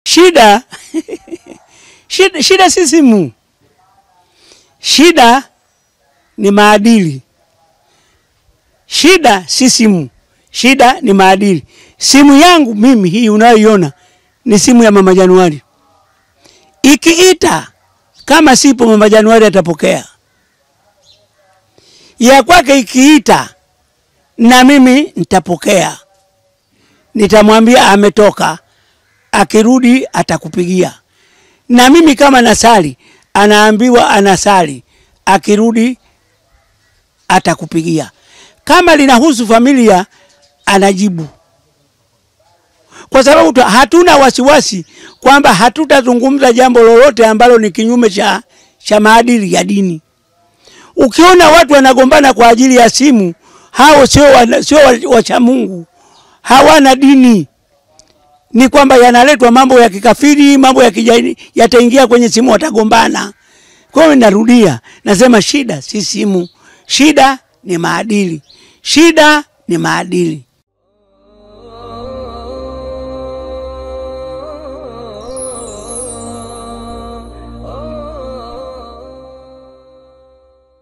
Simu inasingiziwa tu ,Tatizo liko hapa - Mzee Yusuph Makamba
Tulipata nafasi ya kuzungumza na Mzee Makamba,moja ya maswali tuliyomuuliza ni Ndoa nyingi siku hizi zinavunjika sababu ya Simu,Kulikooni.

Haya ndio yalikuwa majibu yake